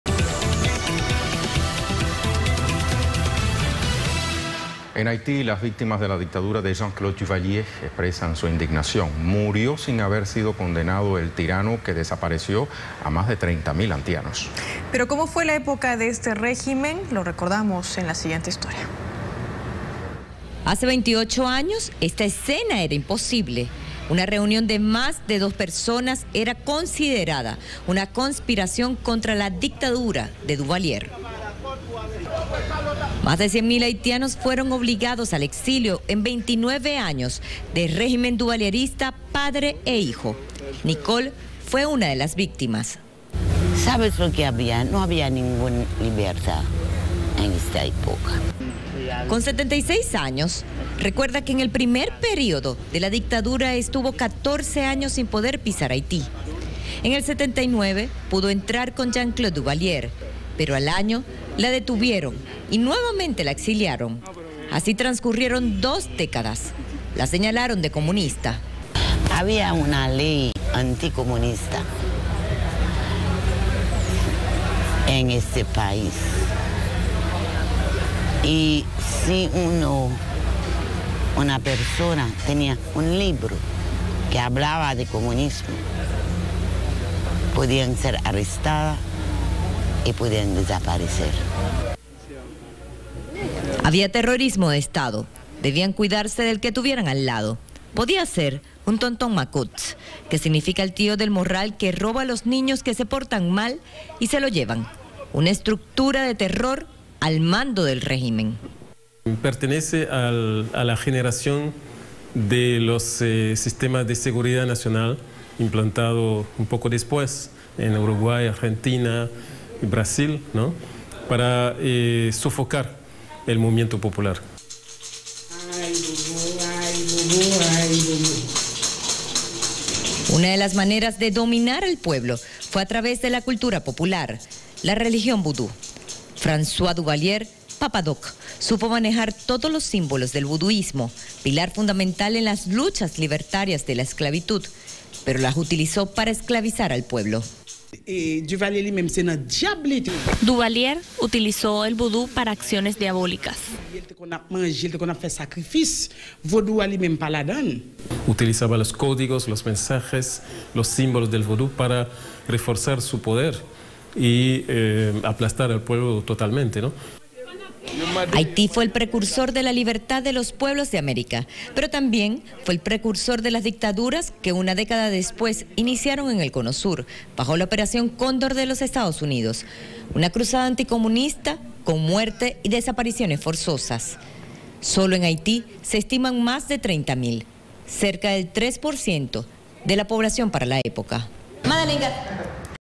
Terror under Jean Claude Duvalier's dictatorship in Haiti
In Haiti, victims of Jean Claude Duvalier's dictatorship in which 30,000 people disappeared, express their outrage that the tyrant died without having been sentenced. teleSUR.